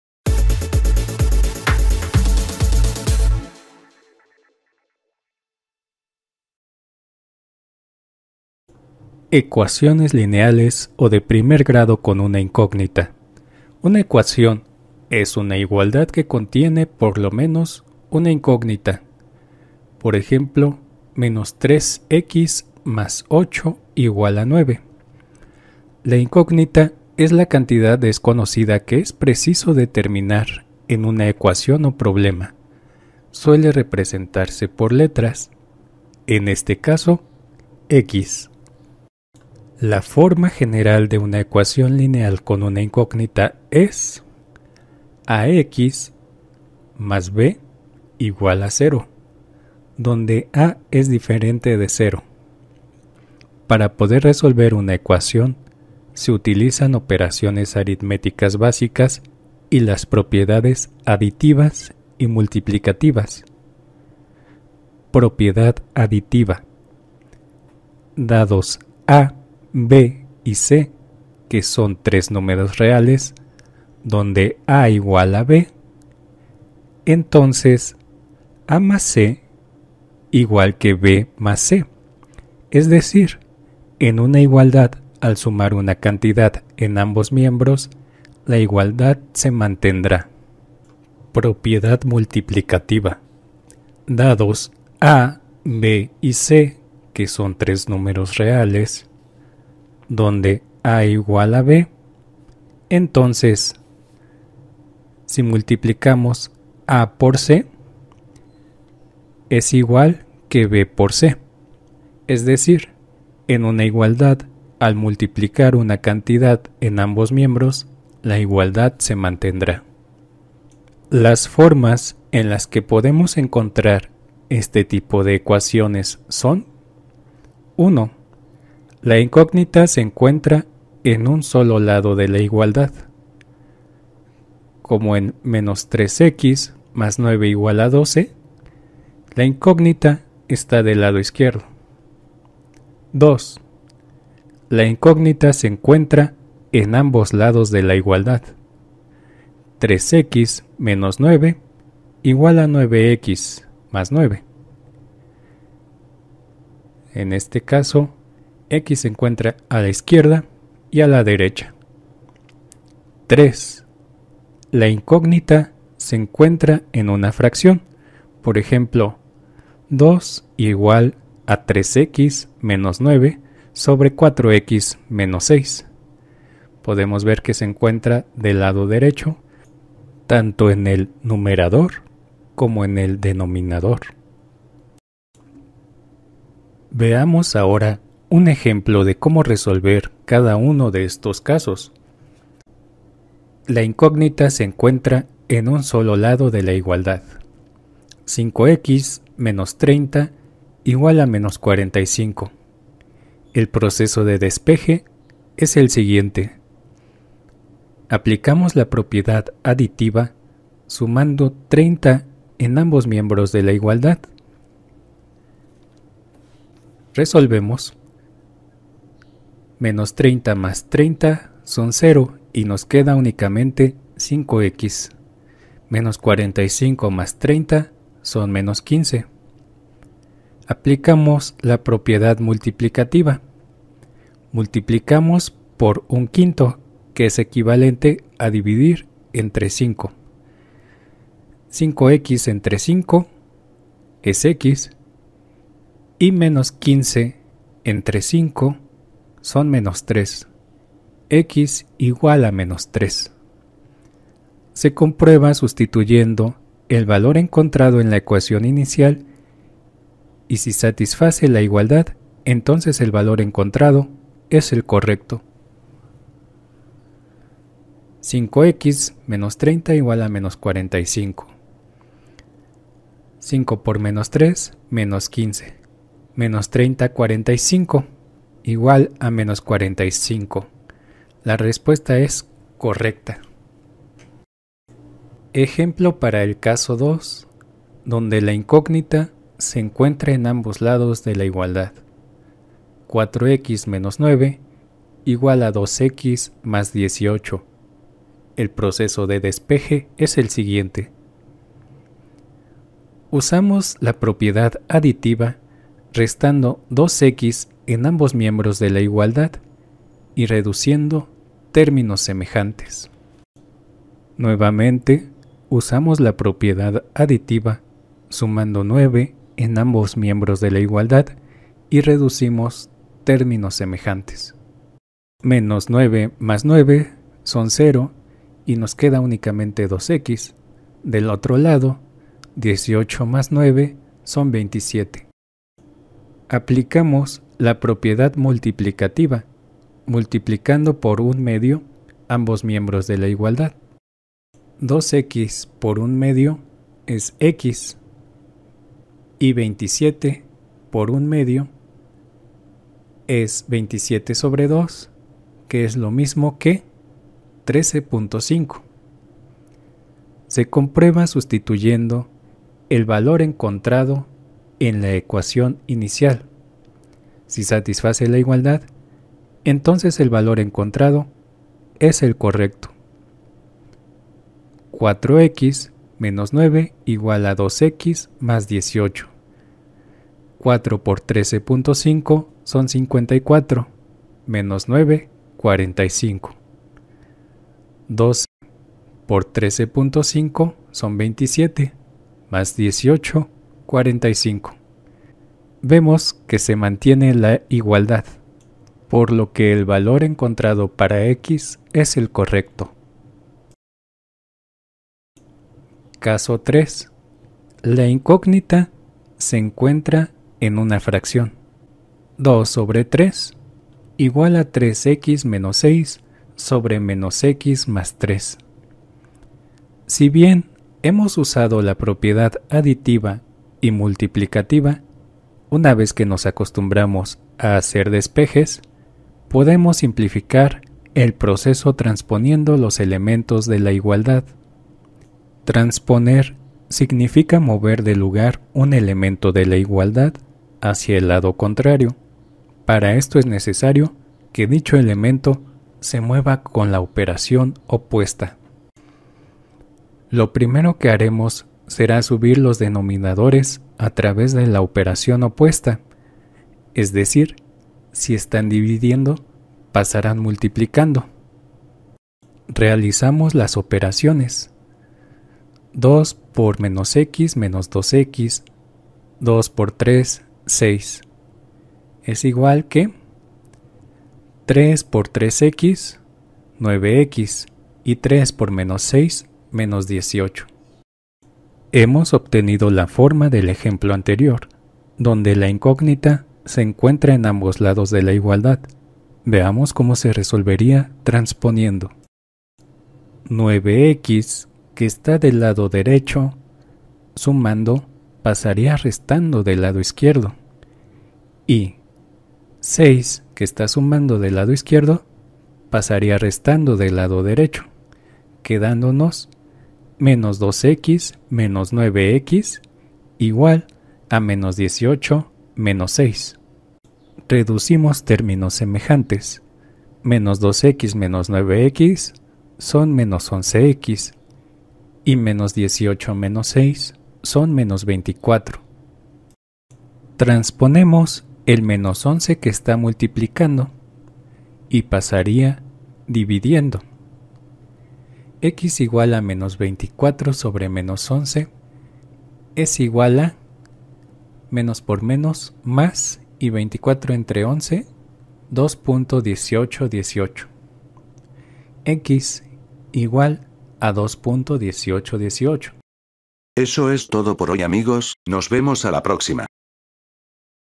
Cómo resolver ecuaciones lineales o de primer grado
En este video vemos la solucion de diferentes casos de ecuaciones lineales o de primer grado.